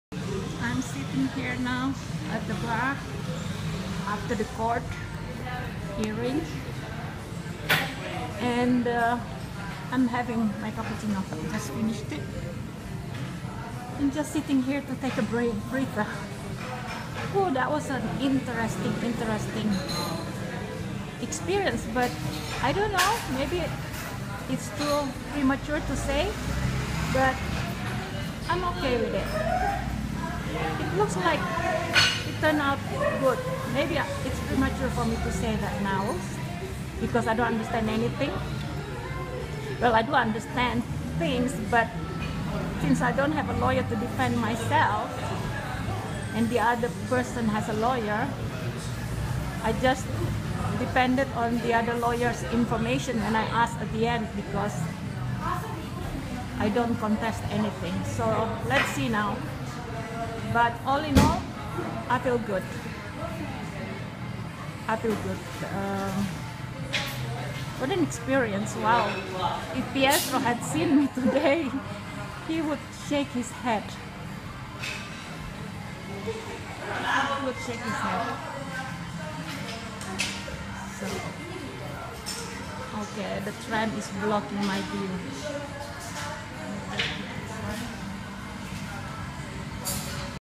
After the Court 1
Just stopping on the way home, at a Bar/Cafe not long after and just reflecting while doing people watching, one of my favourite pastimes living here in Milan, Italy.